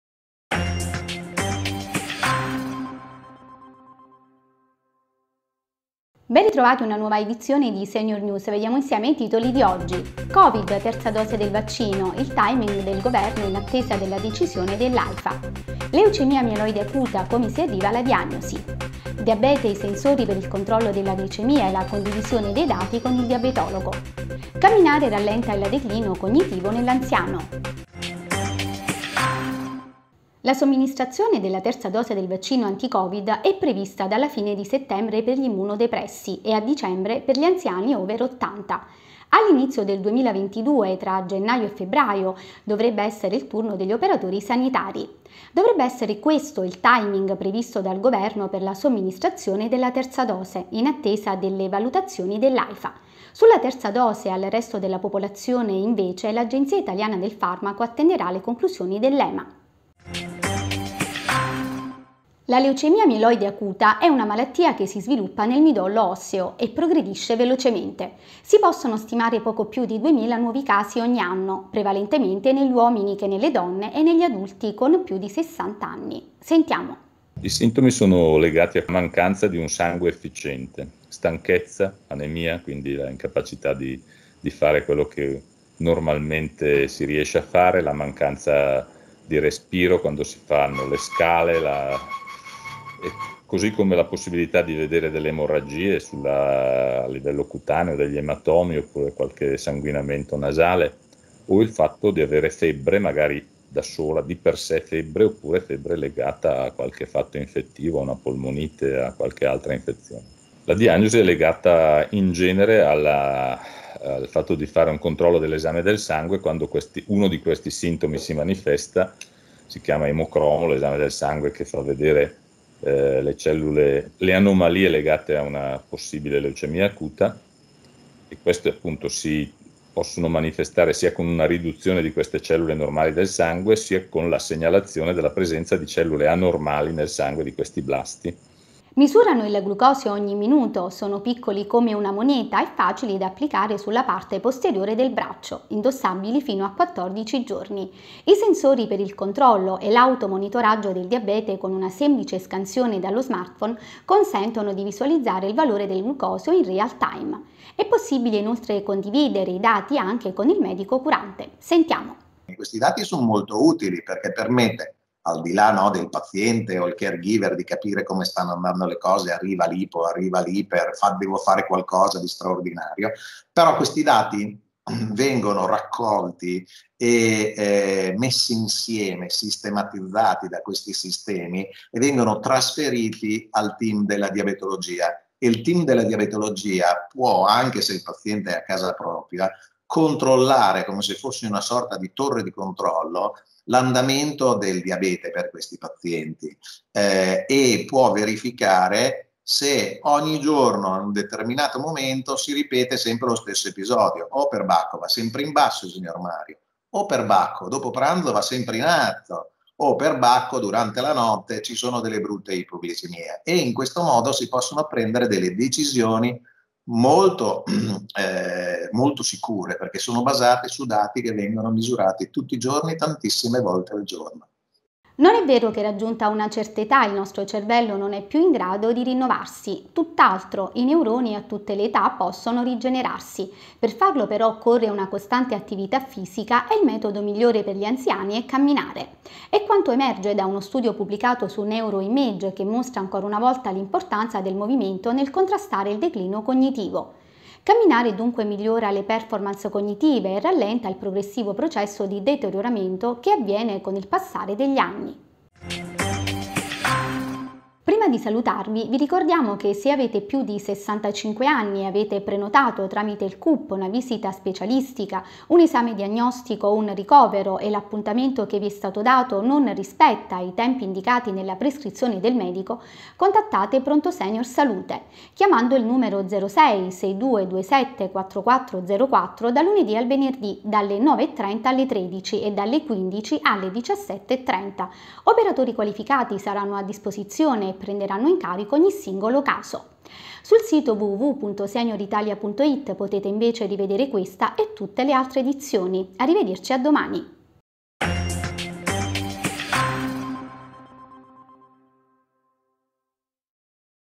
SENIOR NEWS 09/09/2021: COVID, TERZA DOSE VACCINO/LEUCEMIA MIELOIDE ACUTA/DIABETE/DECLINO COGNITIVO
SENIOR NEWS 09/09/2021:
 - COVID, TERZA DOSE DEL VACCINO: IL TIMING DEL GOVERNO IN ATTESA DELLA DECISIONE DELL'AIFA
 - LEUCEMIA MIELOIDE ACUTA, COME SI ARRIVA ALLA DIAGNOSI, INTERVISTA A: PROF. GIUSEPPE ROSSI - CONSULENTE DELLA DIVISIONE DI EMATOLOGIA SPEDALI CIVILI DI BRESCIA
 - DIABETE, I SENSORI PER IL CONTROLLO DELLA GLICEMIA E LA CONDIVISIONE DEI DATI CON IL DIABETOLOGO, INTERVISTA A: PAOLO DI BARTOLO - PRESIDENTE AMD ASSOCIAZIONE MEDICI DIABETOLOGI
 - CAMMIANRE RALLENTA IL DECLINO CONGNITIVO NELL'ANZIANO